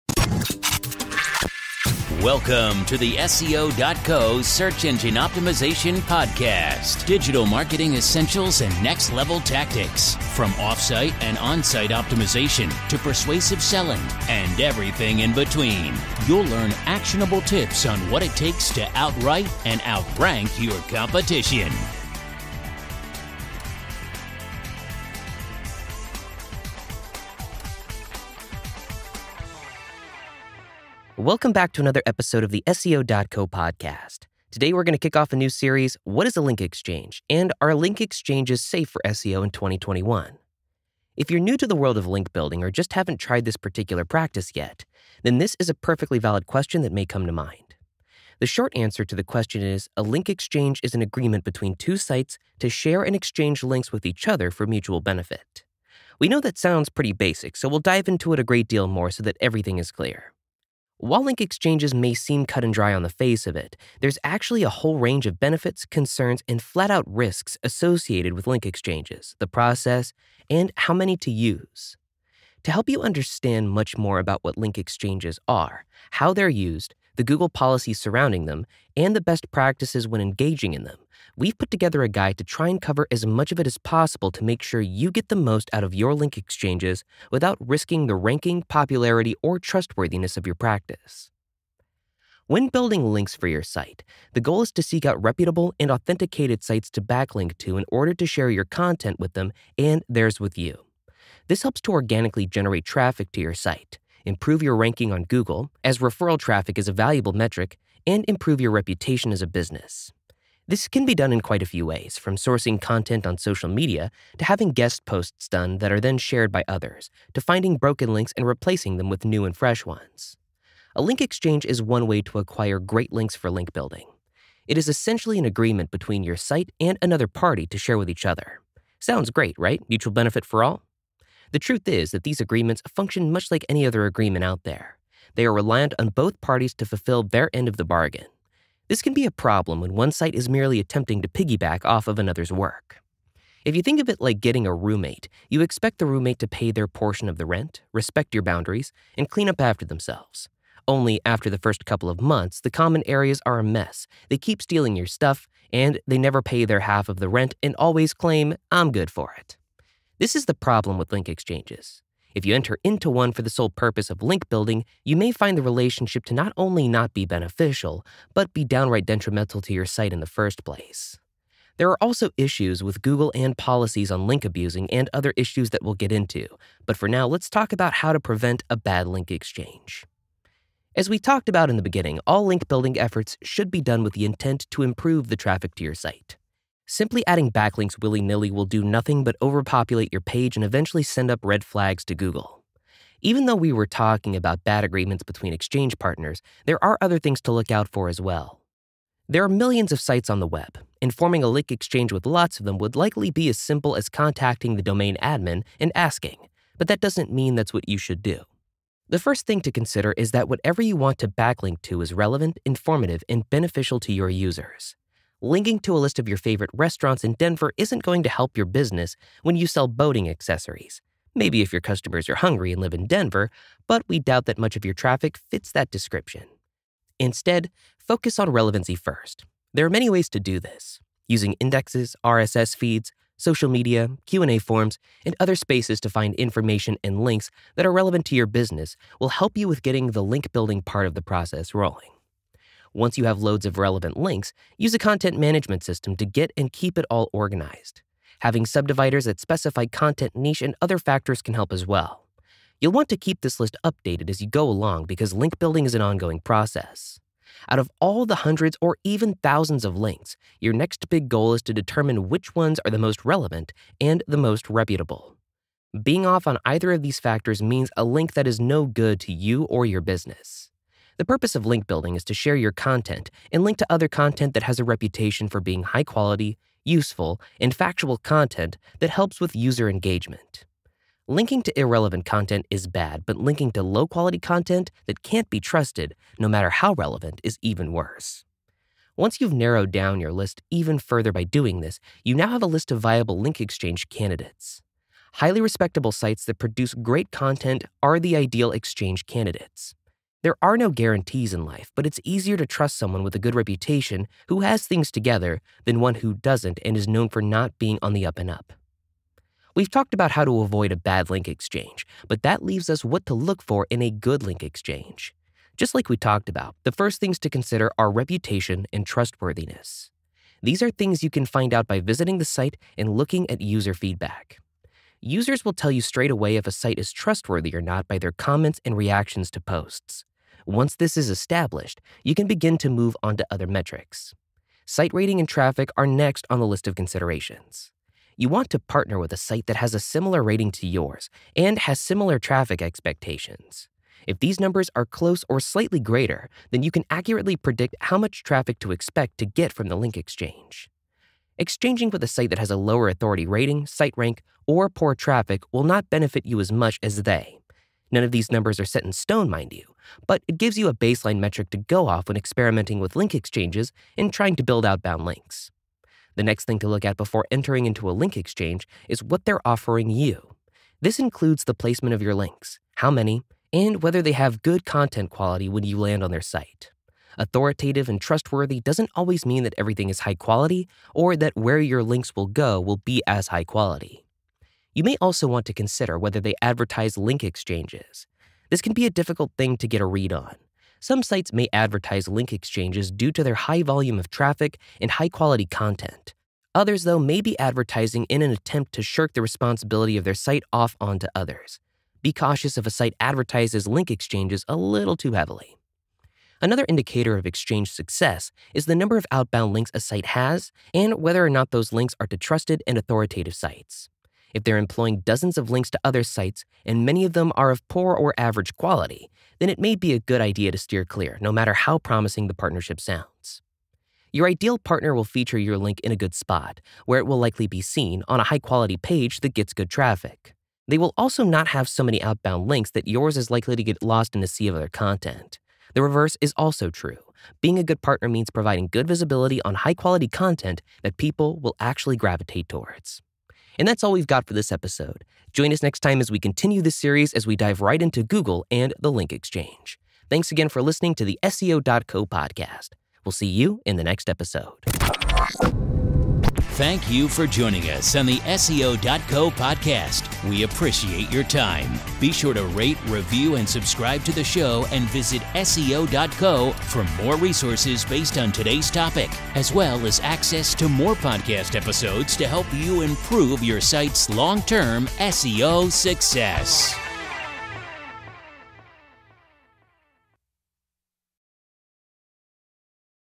#500: What is a Link Exchange in SEO, Episode 1
A link exchange is a mutual agreement between webmasters of equal standing to trade links. Link exchanges are considered an integral part of search engine optimization (SEO) efforts for many individuals and businesses. Ultimately, this proves that link exchanges play a critical role in search engine rankings – and in today's business climate – they are almost essential.

 This is the big question among webmasters today. With the continuously growing trend of link exchange, people are unsure whether to join in or not. This podcast will give you the facts about link exchanges and hopefully some new insights on the topic.

 SEO // PPC // DEV // LINK BUILDING